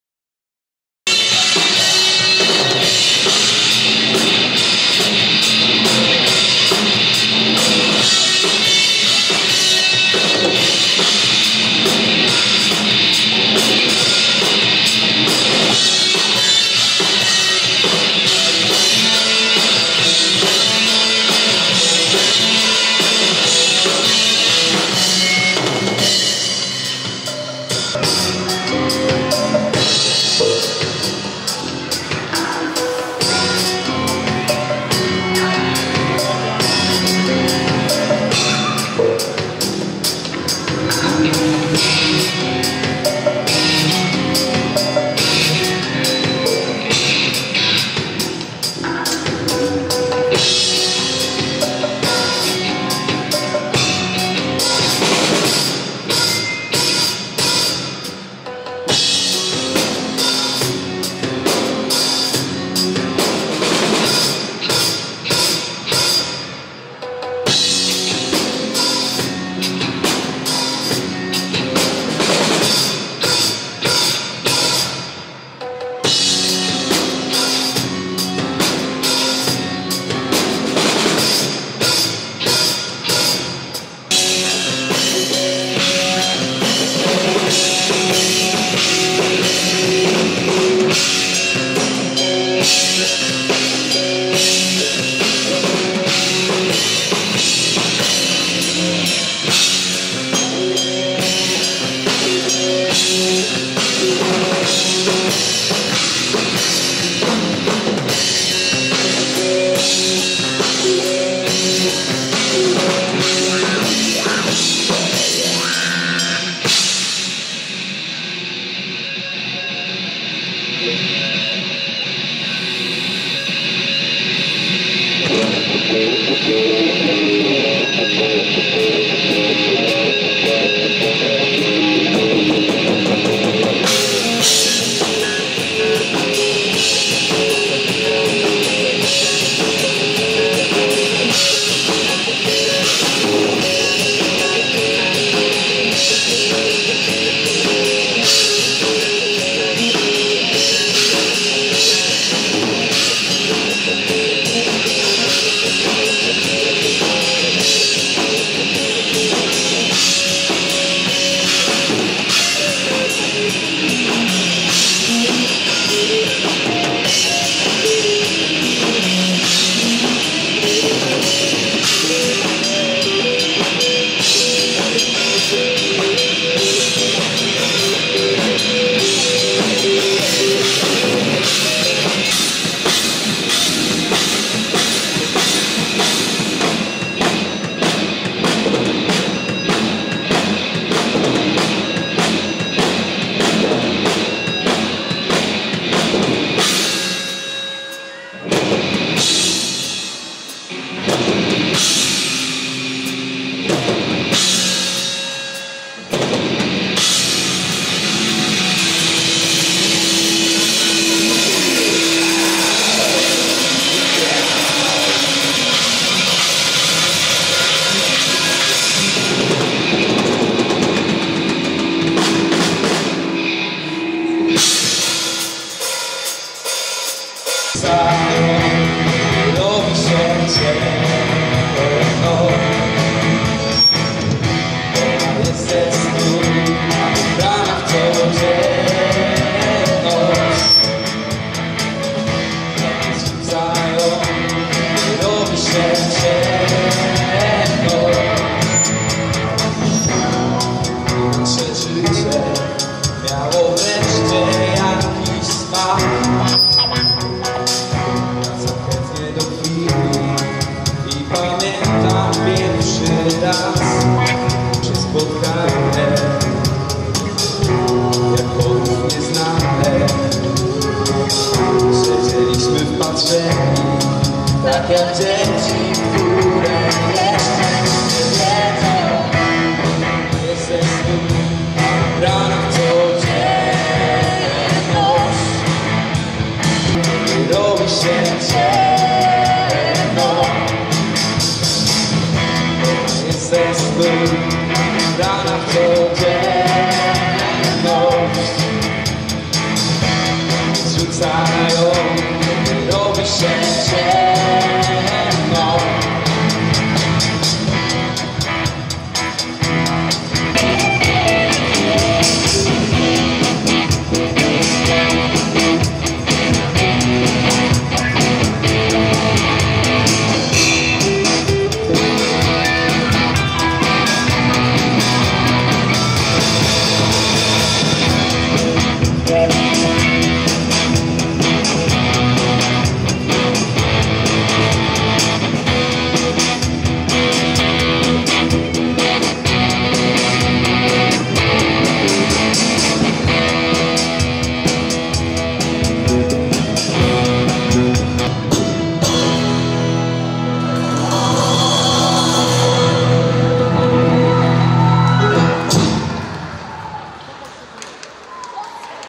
„MEGARAŻ” i „RedBand” zagrali z Wielką Orkiestrą Świątecznej Pomocy
Koncerty, Bieg Wielkiej Orkiestry Świątecznej Pomocy, licytacje, festyny wszystko to w wielkim finale 10 stycznia w Grudziądzu.